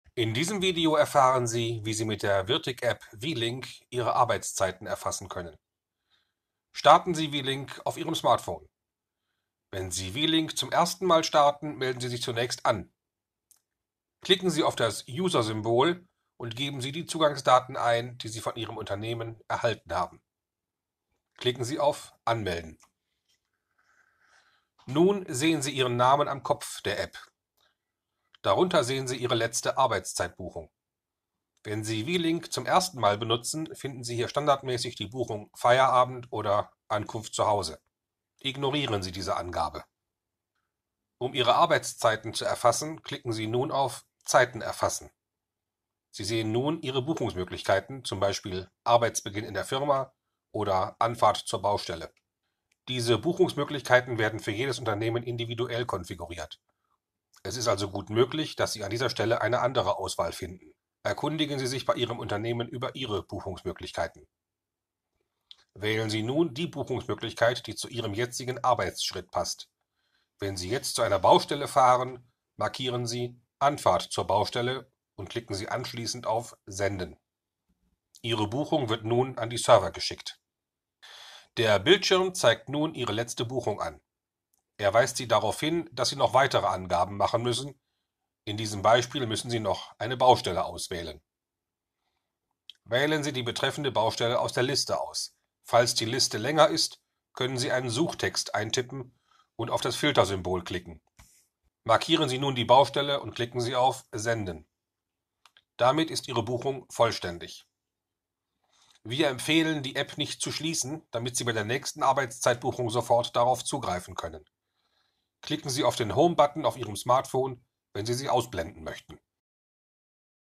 vLinc 21 - Schnelleinstieg
So erfasst du deine Arbeitszeiten mit der virtic-App vLinc21